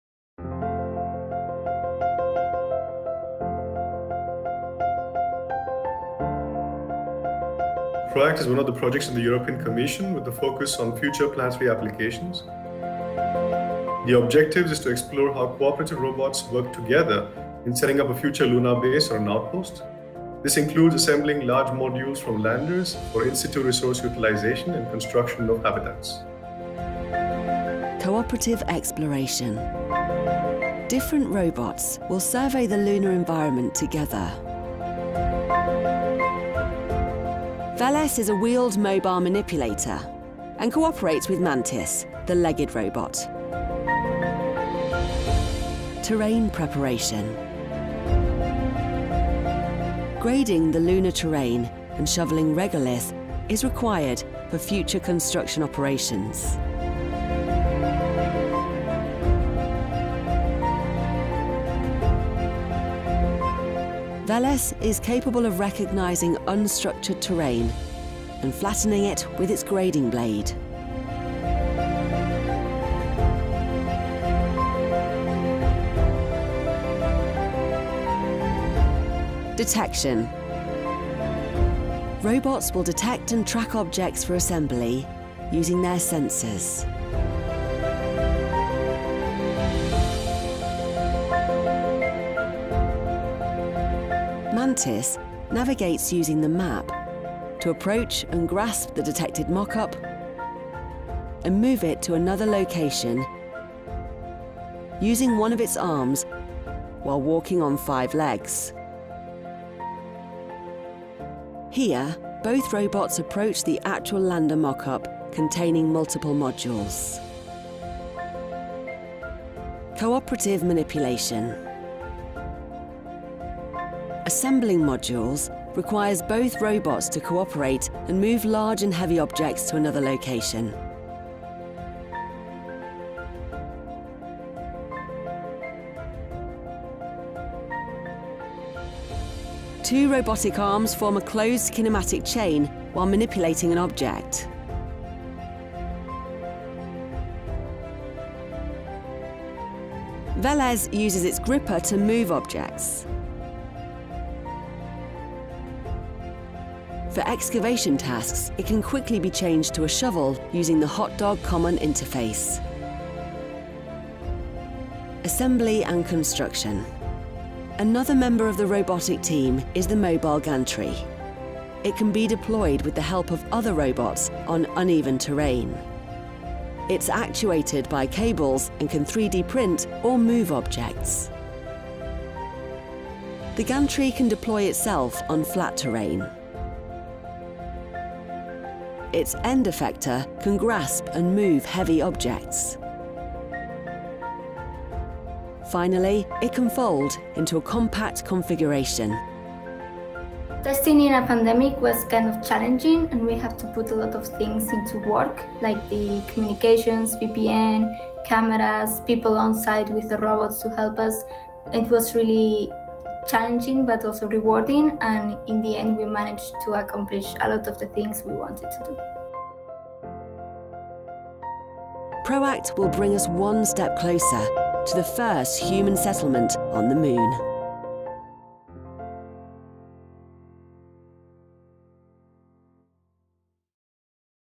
PRO-ACT: Power of Robots Collaborating in Space Applications
Video with Project Objectives and Demonstration Outcomes.

PRO-ACT aims to demonstrate multi-robot collaborative planning, motion and manipulation capabilities in a future lunar construction context. More information on:

Platform models
-VELES (SBŁ-PIAP/PIAP SPACE)
-Mantis (DFKI)
-Mobile Gantry (AVS)

The PRO-ACT Consortium
-Space Applications Services, Belgium
-AVS,  Spain
-Deutsches Forschungszentrum für Künstliche Intelligenz (DFKI),  Germany
-GMV, Spain
-Laboratoire d’Analyse et d’Architecture des Systèmes (LAAS), France
-La Palma Research Centre S.L. (LPRC), Spain
-Industrial Research Institute for Automation and Measurements (PIAP-Space), Poland
-City, University of London, Great Britain
-Thales Alenia Space, United Kingdom

Video directed and edited by Andres Martin Barrio.
This project is co-funded by the European Commission under the Horizon 2020 Framework. Grant number 821903